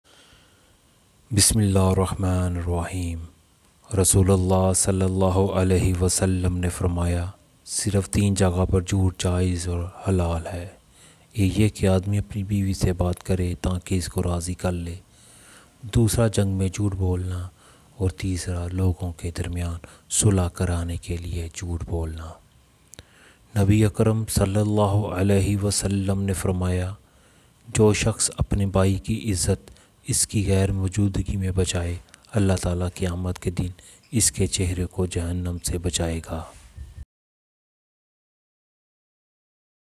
Hadees in urdu | teen jaghon par jhoot jaiz ha | islamic short video
The Messenger of Allah (peace and blessings of Allah be upon him) said:
 Lying is permissible and halal in only three places,
 One is for a man to talk to his wife
 to satisfy him,
 The second is lying in war and
 The third is to make peace between people
 To lie."
The Prophet (PBUH) said:
 "He who honors his brother
 (in his absence) save
 His face on the Day of Resurrection
 will save him from hell".

Hadees e Mubarak status
Daily Hadees
Islamic status